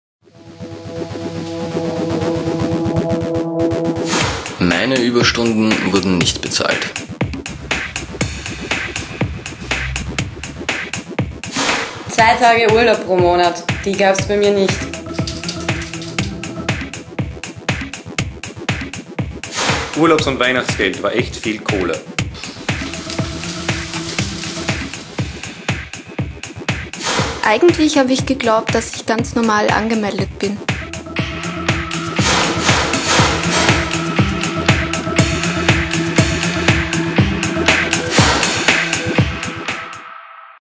Dein Ferienjob - Deine Rechte
Damit sich der Ferienjob auszahlt!
Viele SchülerInnen und StudentInnen arbeiten im Sommer. Weil es ihr Lehr- oder Studienplan so vorschreibt, oder weil sie einfach das Geld brauchen. Leider nutzen einige ArbeitgeberInnen das aus! Damit du bekommst was dir zusteht, haben wir hier die wichtigsten Infos zusammengefasst.